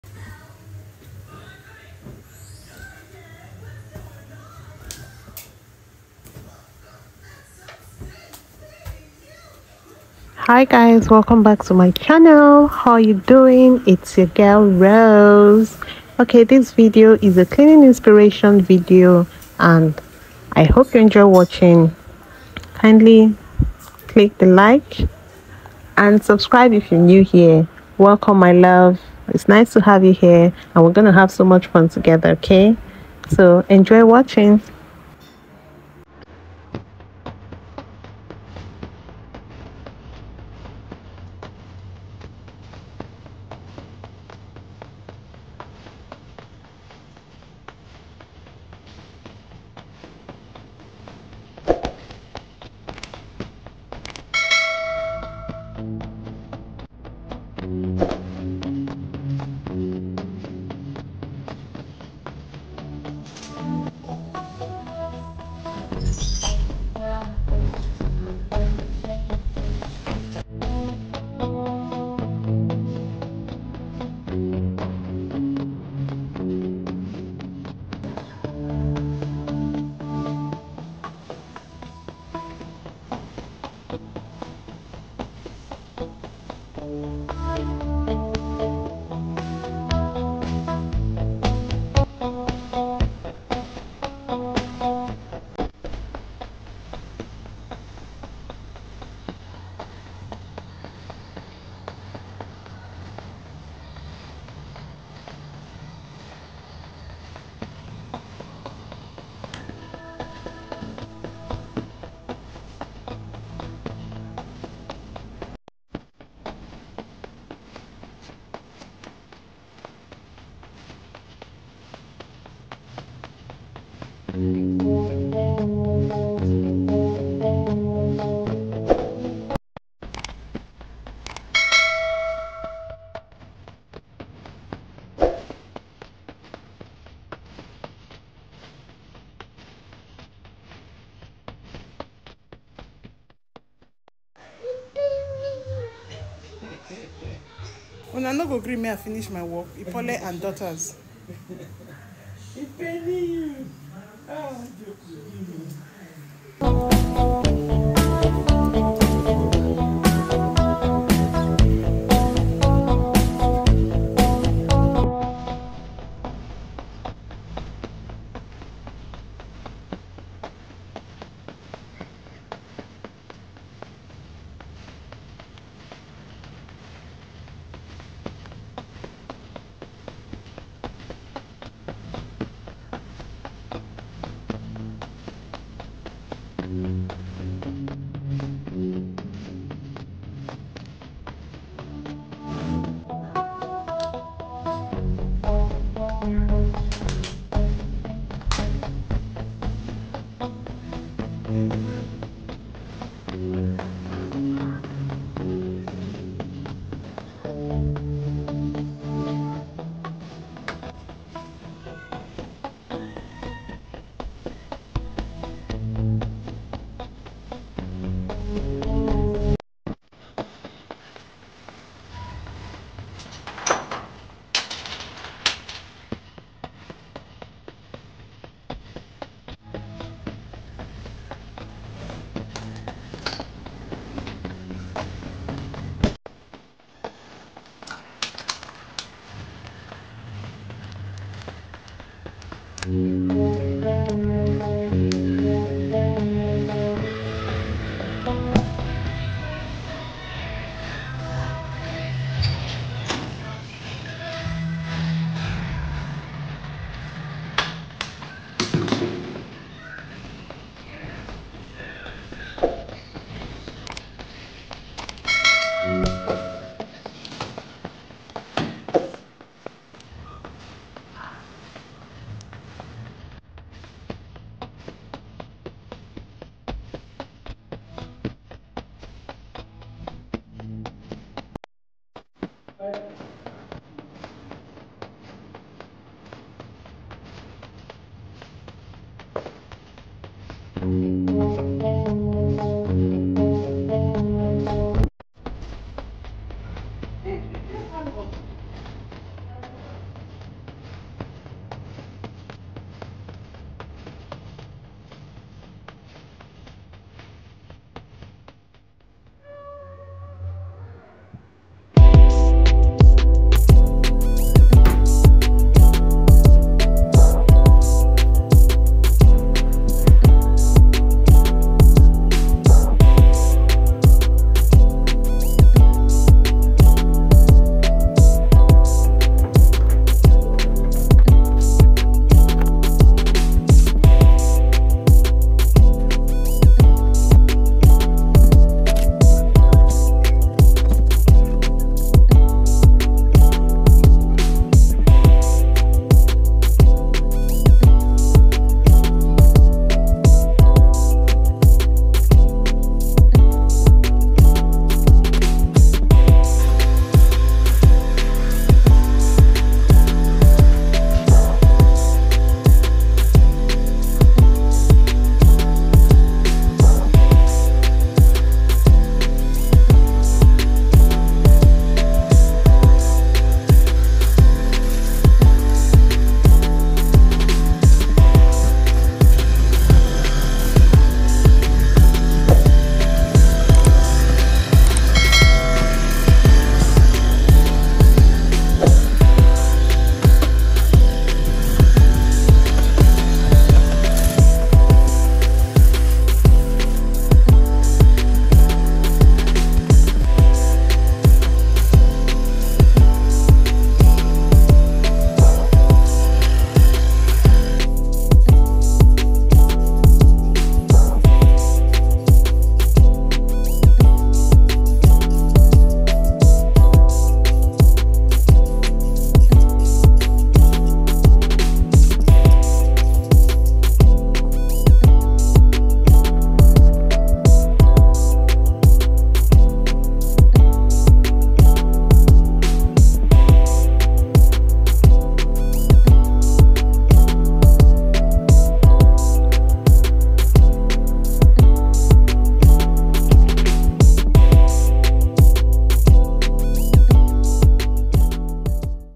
CLEANING MOTIVATION / MY CLEANING ROUTINE
Hello lovelies, I hope I inspire someone with this video to go and do that cleaning which you have been putting off. Enjoy